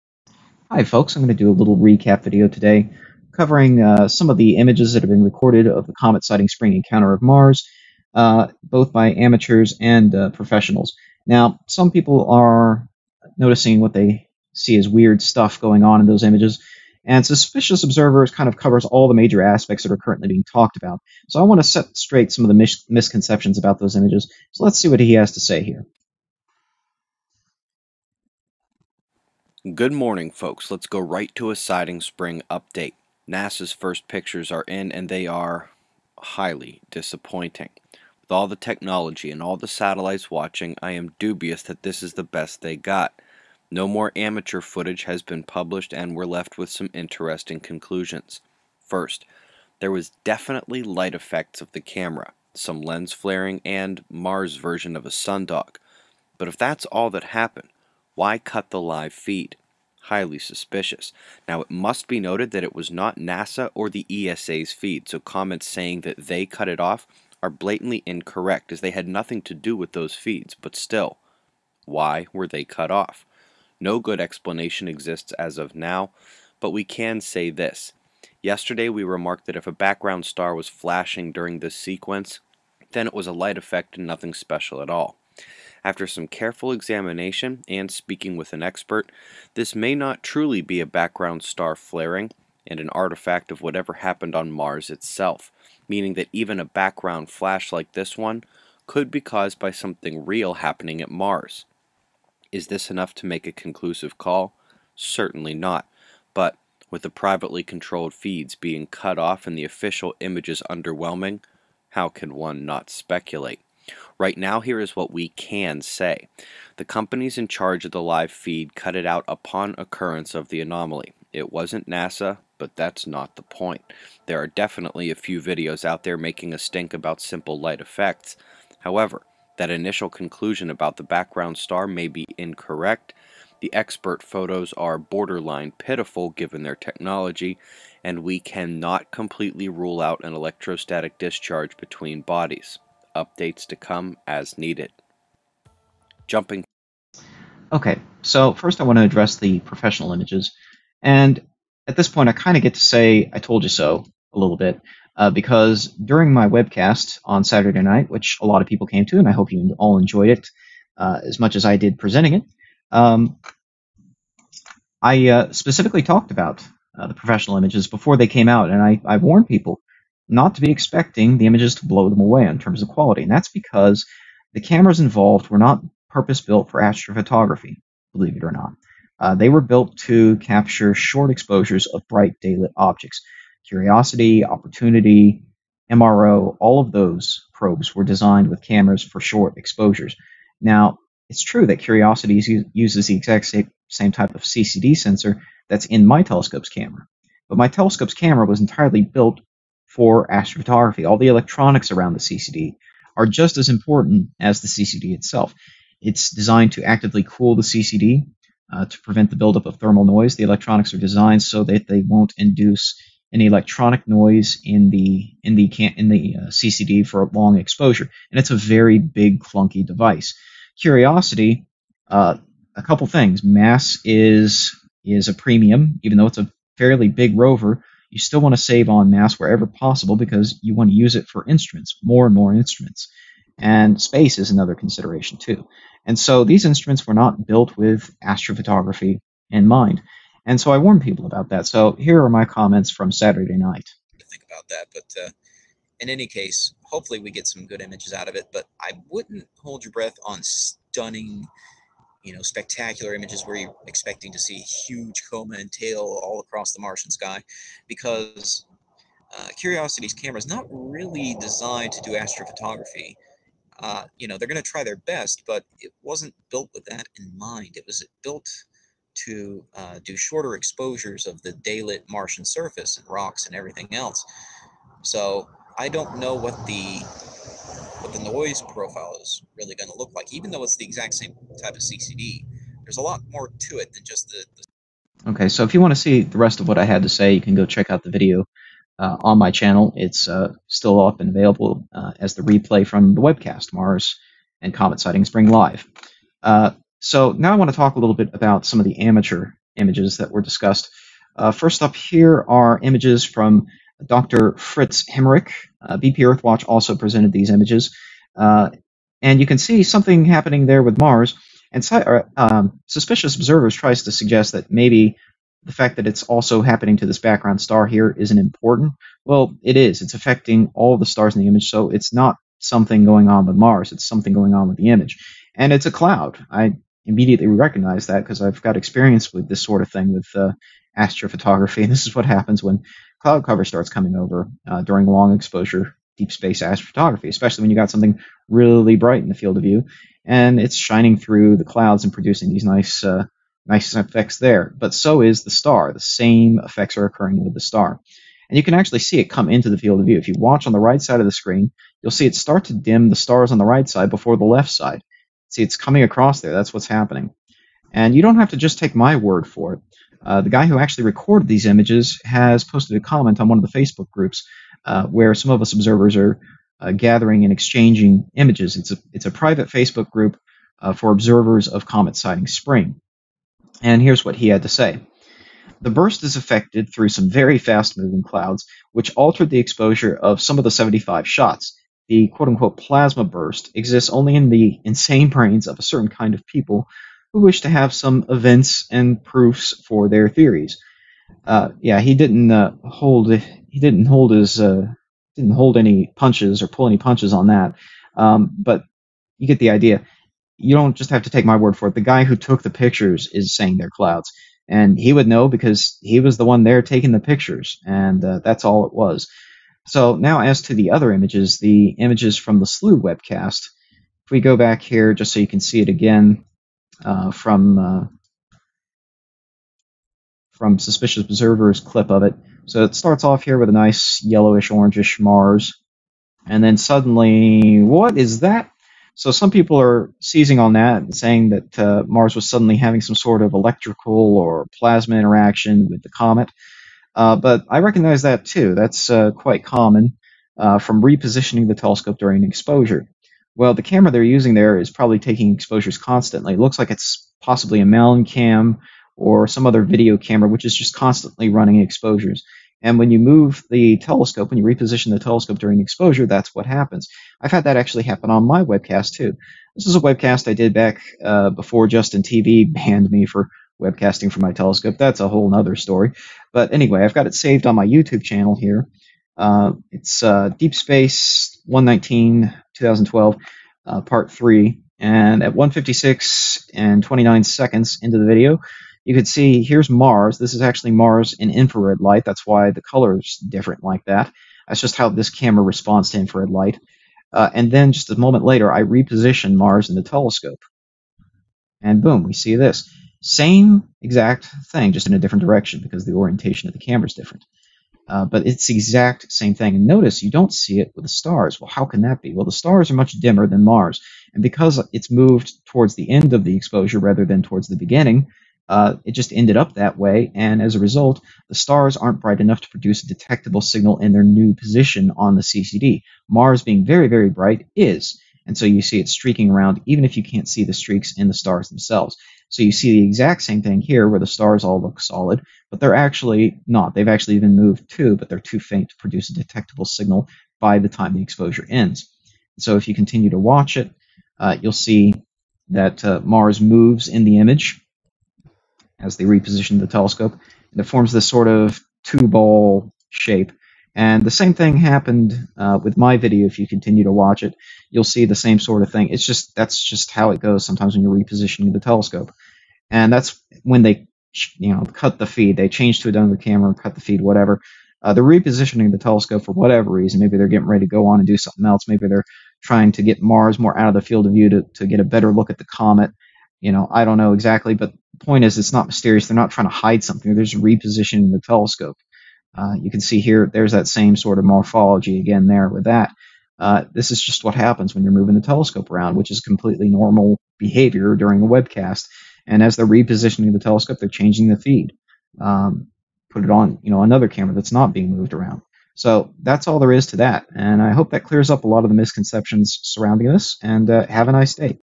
Astronomy Live - Comet Siding Spring Recap - Misconceptions and Anomalies
This video is by Astronomy Live (shared with permission).